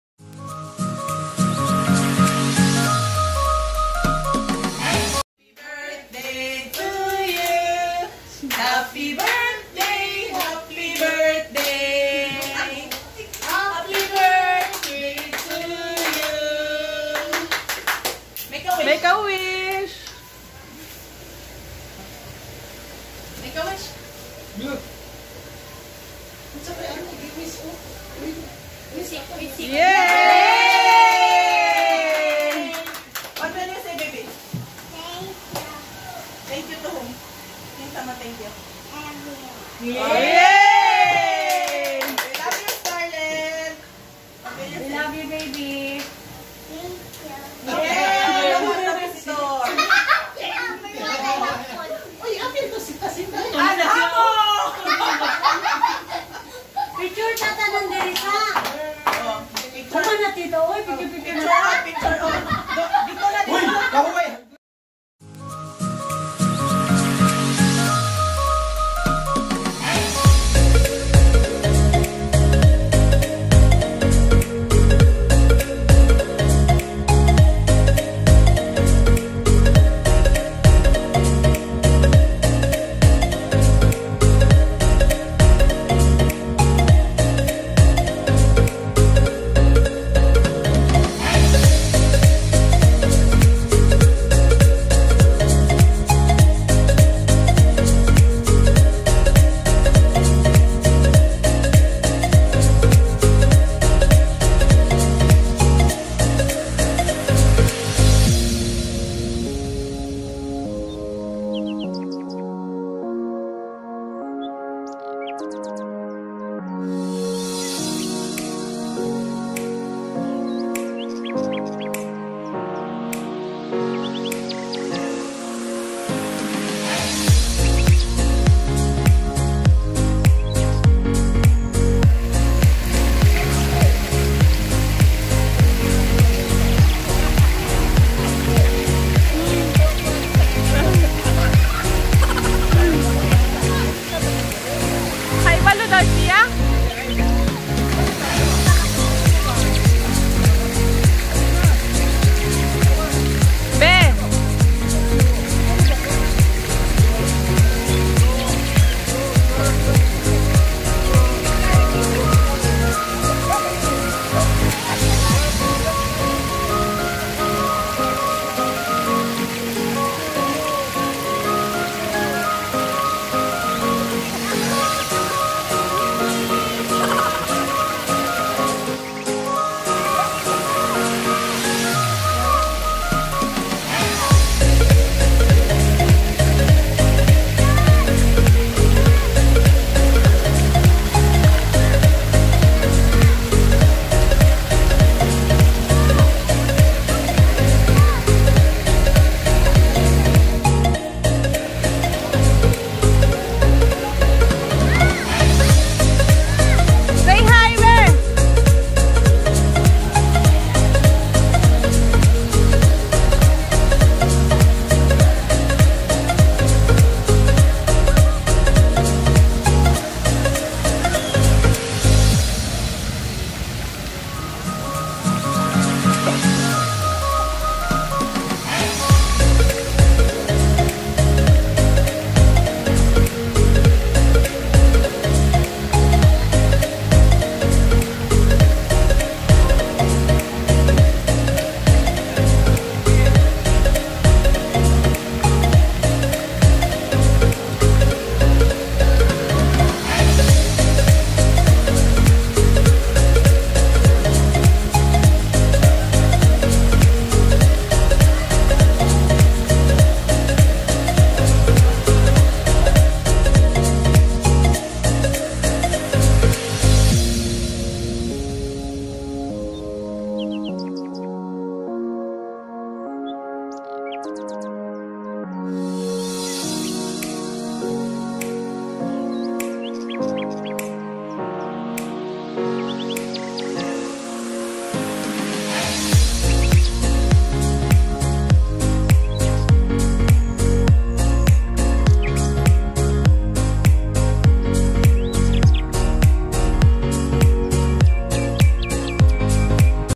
ZOE'S 7TH BIRTHDAY CELEBRATION +SWIMMING TIME at westown lagoon|lucy&zoe vlog
hi guys,i want to share my daugther 7th birthday, simple celebration her wish to makapag swimming,she so happy that her simple wish granted

thank you and godbless you all